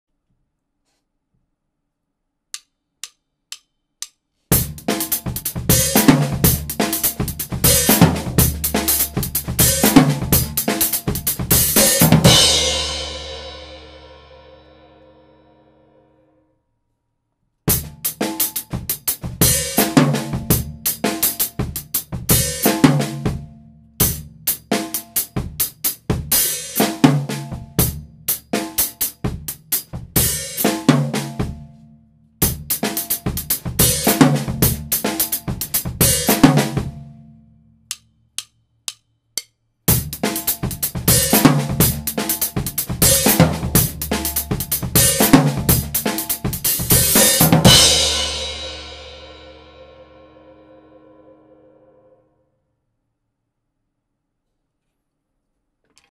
Easy Funk Groove ( Jimmy Branly Style ) - Drum Lesson #178
ジミー・ブランリーの比較的簡単なファンク・グルーヴを真似てみました。